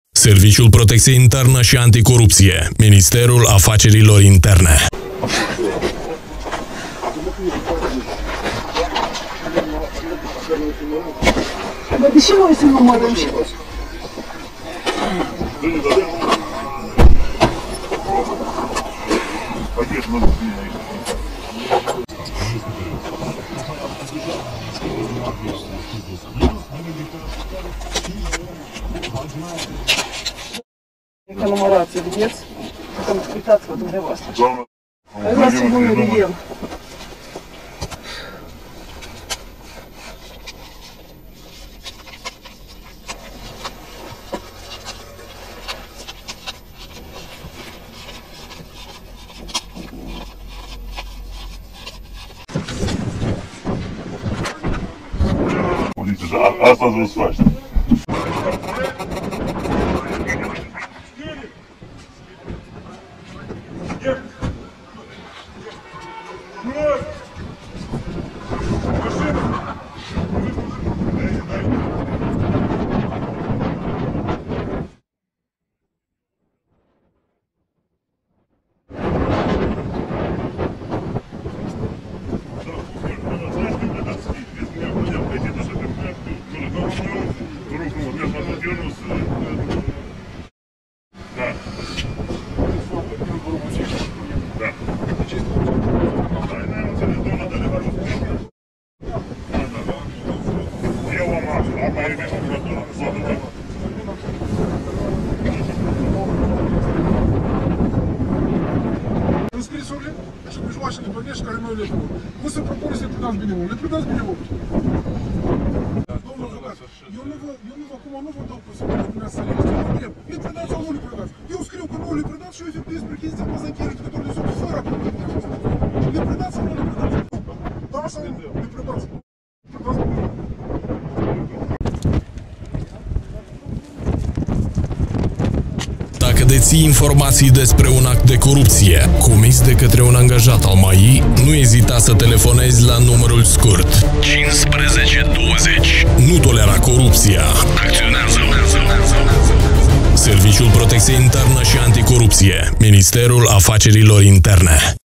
Un bărbat a fost reținut de către ofițerii SPIA în timp ce estorca 600000 de lei
Ofițerii Serviciului protecție internă și anticorupție al MAI, de comun cu Procuratura Anticorupție, au reținut recent, în flagrant delict, un locuitor al capitalei, în timp ce estorca și primea 600 000 de lei de la o cetățeancă.